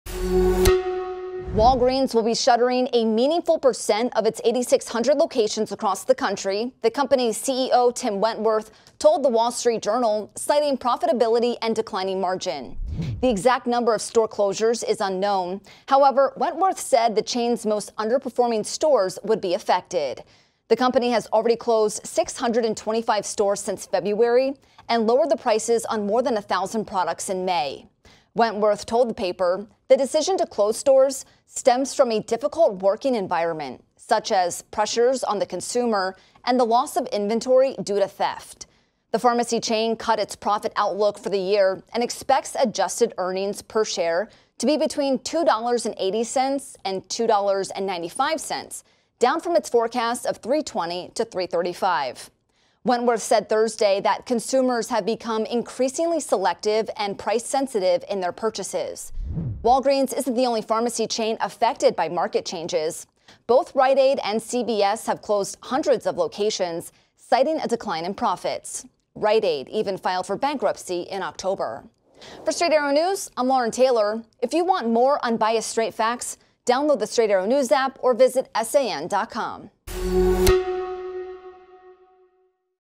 Walgreens to close ‘significant’ number of stores as profits fall
Walgreens announced on Thursday, June 27, that it will close a meaningful percent of its 8,600 locations across the country, according to the company’s CEO, Tim Wentworth. The decision came as the chain faces profitability challenges and declining margins.